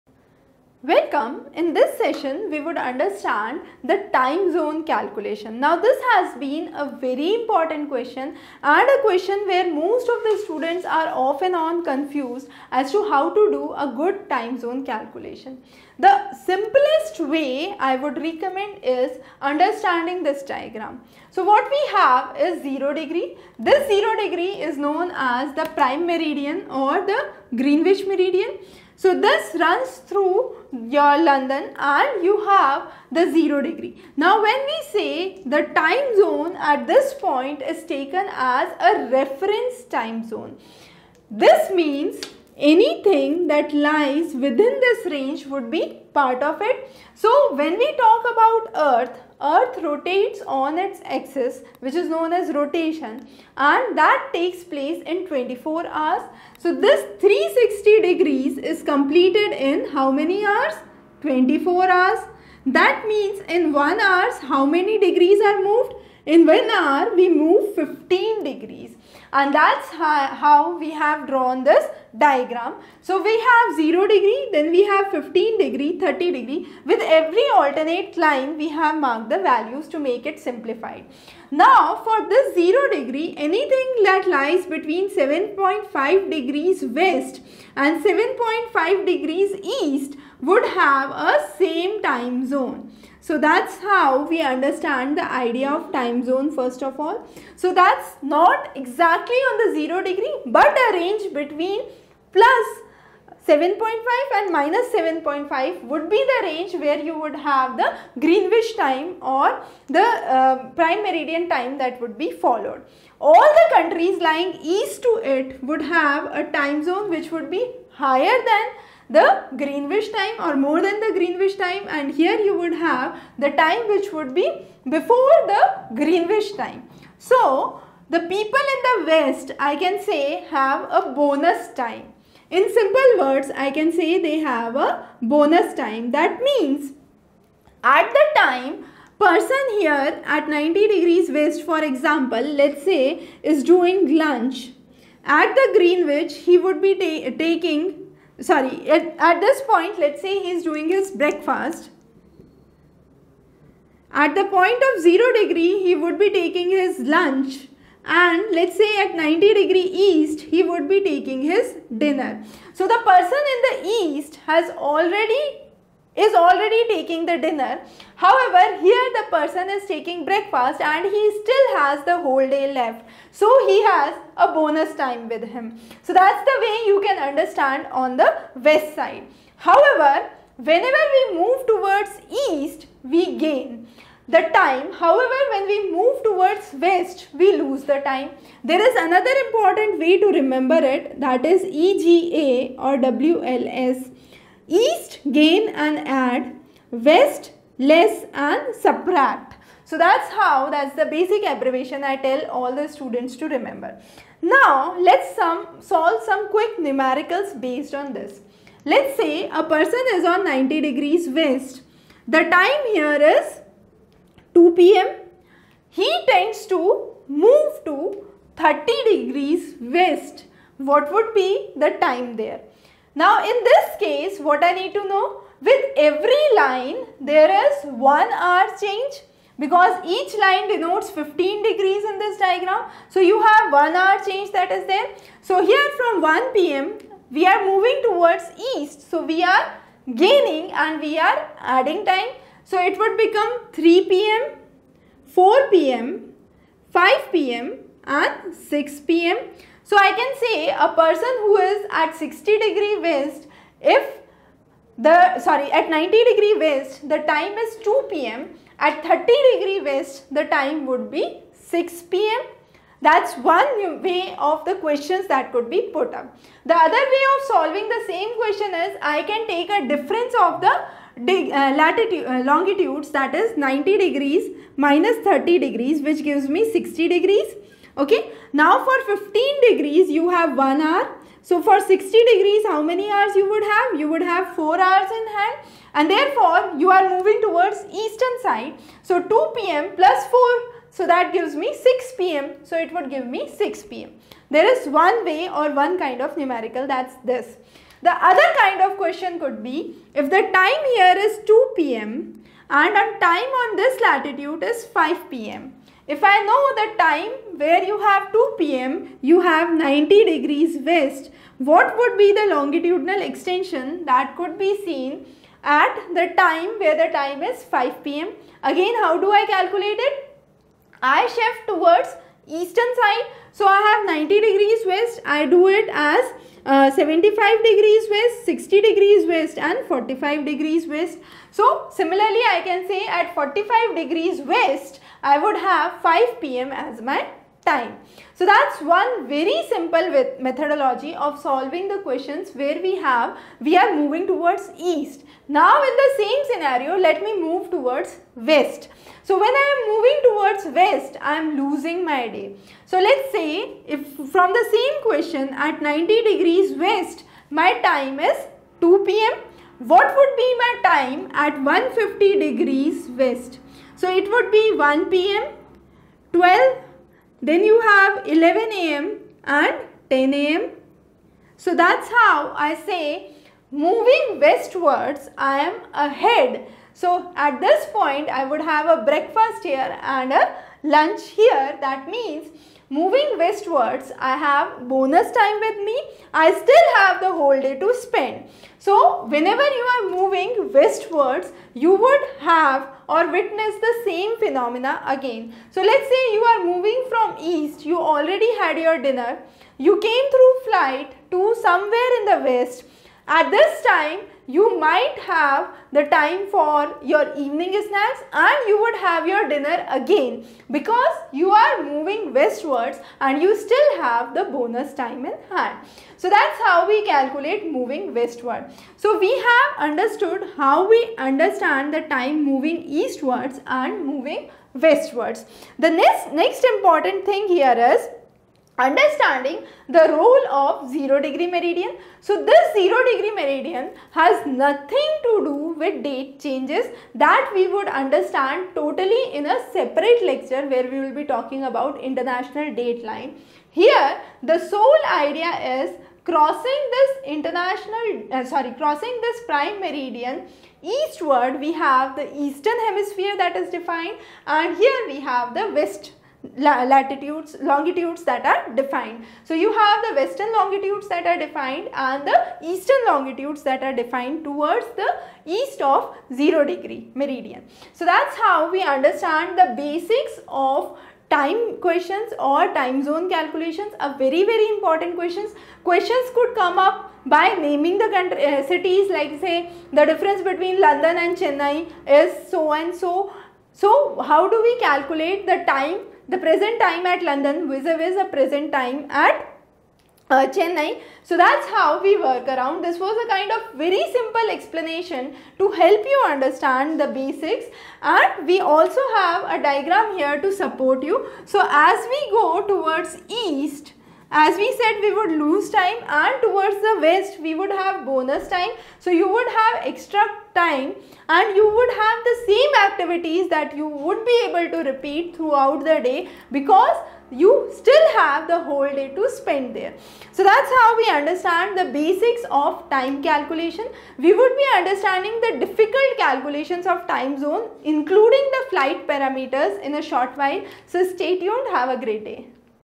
🌎 GMT and Time Zone Calculations (Moving East and West) - Basics (Part 1 of 3)
Dr. Manishika Jain in this lecture explains GMT and Time Zone Calculation (Moving East and West) - Basics (Part 1 of 3)
Chapters:
0:00 Introduction: GMT and Time Zone Calculations (Moving East and West) - Basics (Part 1 of 3)
0:14 Time Zone Calculation
0:25 Prime Meridian
3:15 EGA (East Gain Add)
3:17 WLS (West Less Subtract)
8:23 Going East is Least: Lose Time
8:28 Going West is Best: Bonus Time